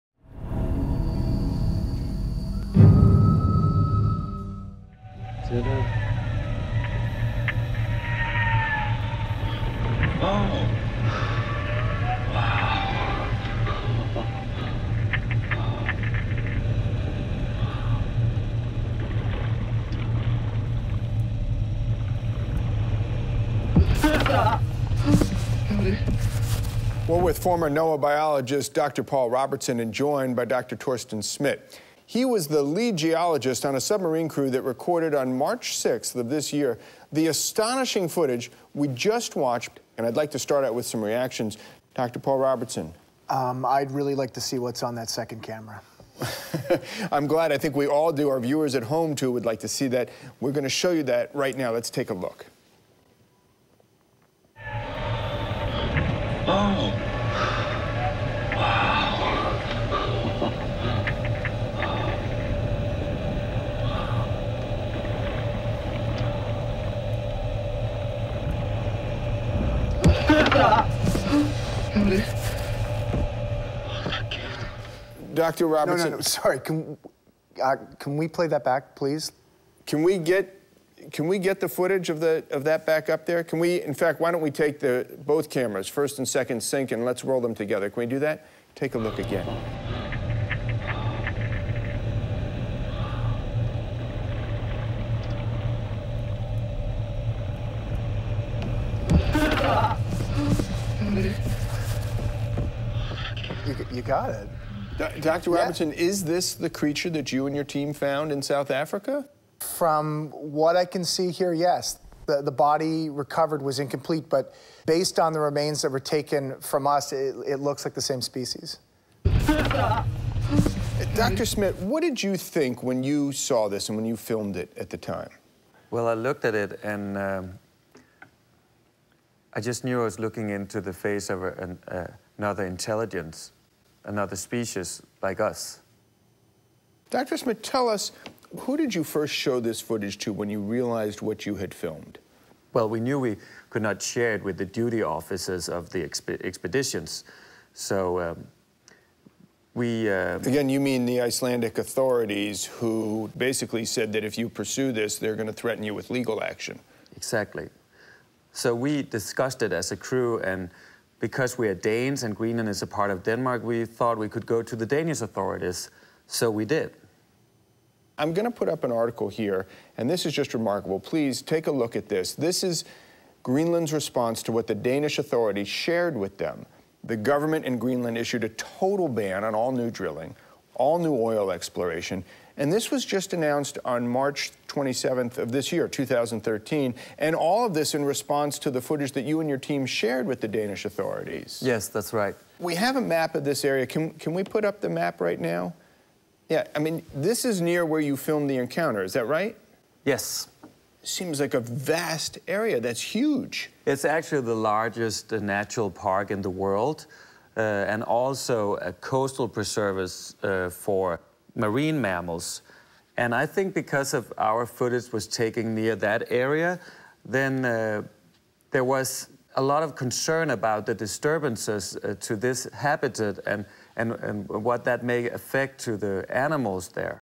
Camera Captures Mermaid on Submersible | Mermaids
A second camera captures what appears to be a Mermaid running into a deep sea diving vessel.